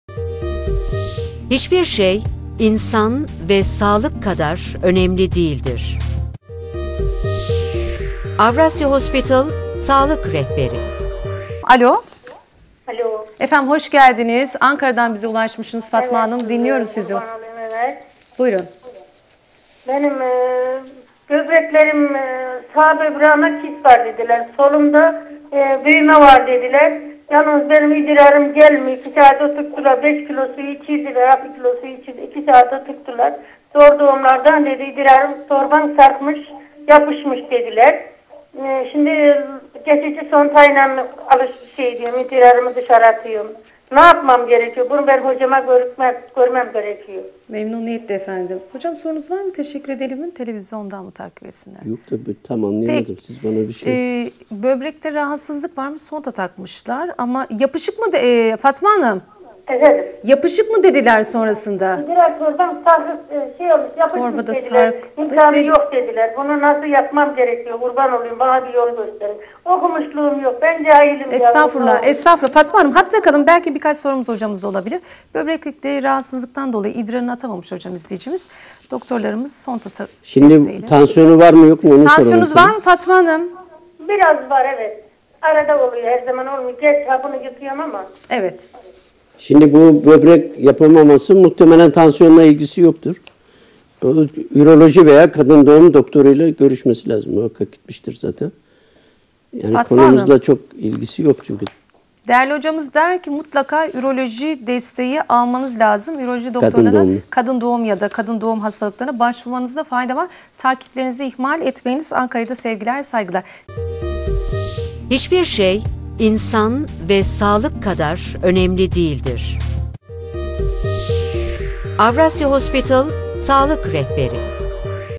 Hastalıklarınız da Takip Tedavilerinizi İhmal Etmeyiniz
Avrasya Hastanesi İç Hastalıkları Uzmanı Dr. Mecdi Hikmet Ergüney '' Hastalıklarınız da Takip Tedavilerinizi İhmal Etmeyiniz  '' hakkında detaylı bilgiler veriyor.